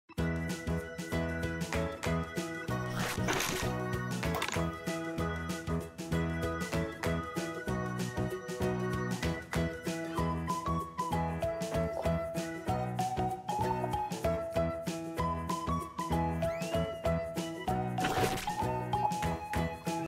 Bite! Beware of Rabbit
▼Original video

▼Shooting place
Okunoshima(Rabbit Island), Tadanoumi Town, Takehara City, Hiroshima Prefecture, Japan

▼Equipment used
camera：Insta360 ONE X
tripod：insta360 All-Purpose Tripod